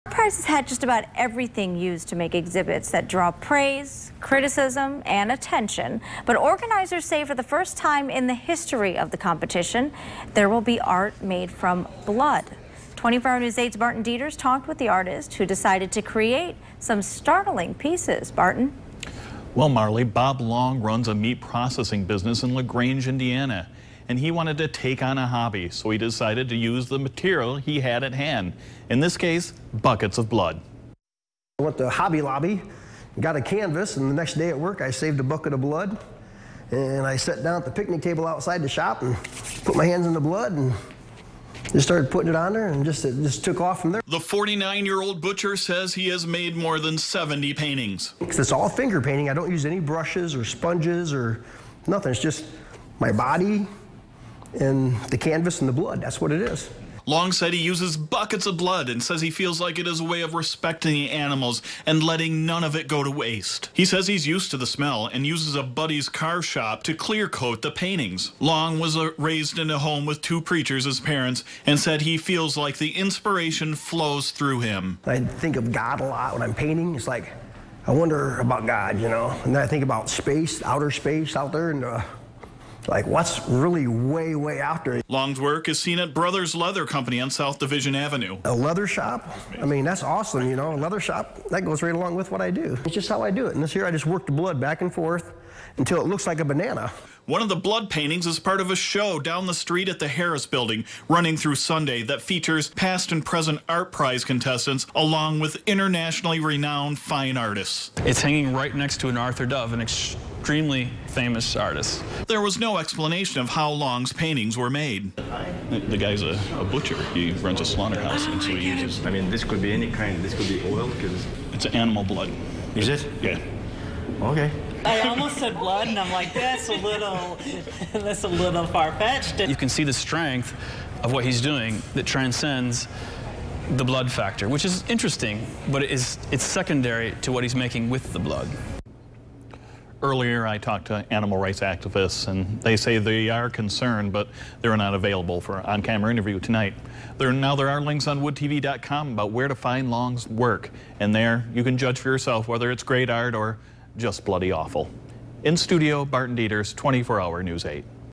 Blood is the medium for unusual ArtPrize entry
Bob Long runs a meat processing business and decided he needed a hobby. He decided to use the material he had at hand buckets of blood. (Sept. 16, 2016)